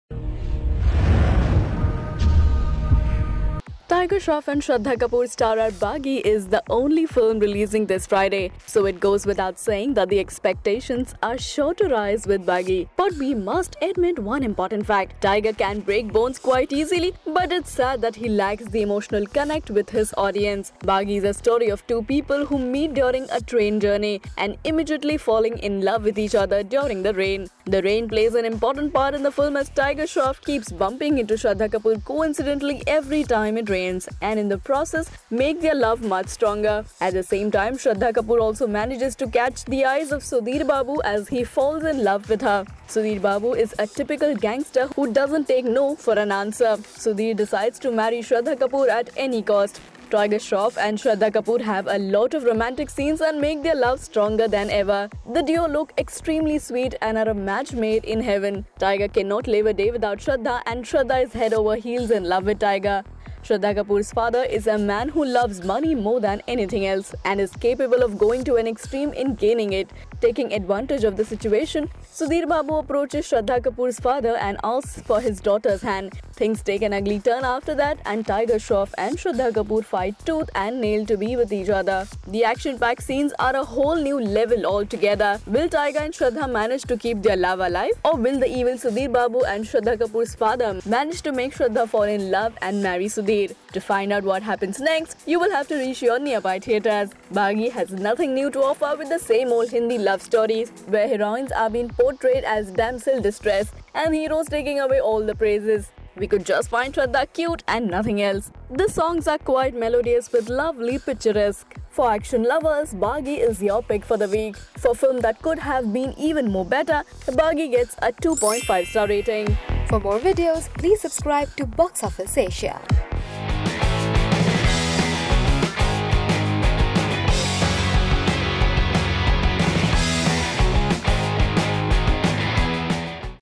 Baaghi Full Movie Review | Tiger Shroff, Shraddha Kapoor | Box Office Asia
Check out the movie review of Tiger Shroff and Shraddha Kapoor starrer 'Baaghi'.